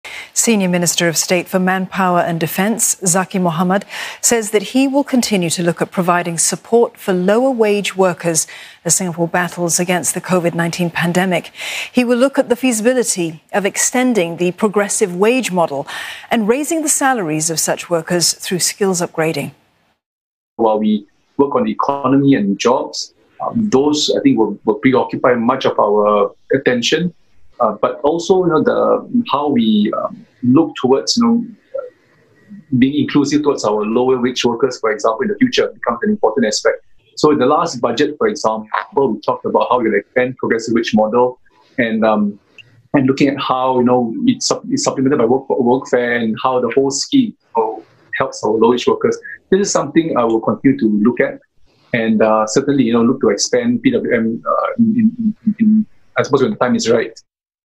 Zaqy Mohamad says he will continue to support lower-wage workers
Senior Minister of State for Manpower and Defence Zaqy Mohamad has said he will continue to look at providing support for lower-wage workers, as Singapore battles the COVID-19 pandemic.